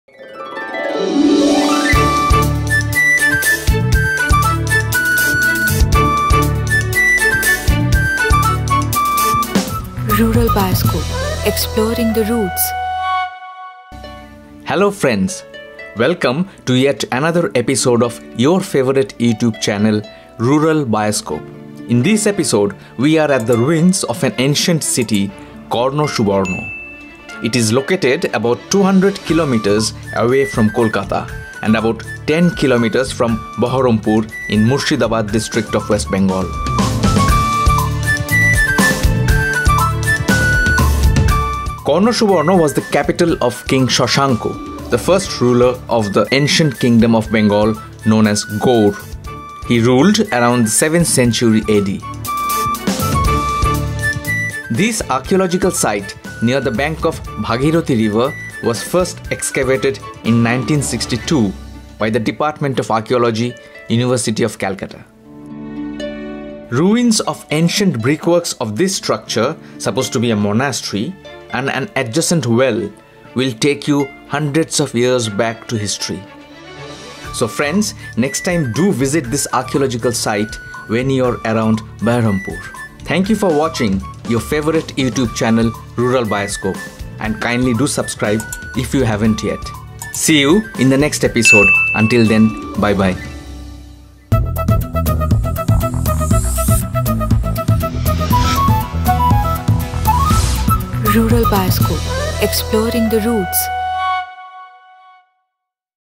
Visit KarnaSubarna, Capital of King Shashanka, About 200 Kms from Kolkata (Special Ep: 16)
This episode of Rural Bioscope was shot at the ruins of an ancient city ‘KarnaSubarna’, the Capital of King Shashanka. It is about 200 Kms. from Kolkata and about 10 Kms. from Berhampore in Murshidabad district of West Bengal.

This Archaeological Site was first excavated in 1962, by a team of the Department of Archaeology, University of Calcutta.

Sight of the remains of ancient brick-works of the structure, supposed to be a monastery, is an experience of re-visiting History.

Special Thanks         : Mr. Rahul Biswas

Cameraperson          : Mr. Biswajit Chanda

Transport                   : Mr. Nantu Paul

Executive Producer  : Mrs. Sangita Sarkar

Creative Director      : Mr. Supurna Sarkar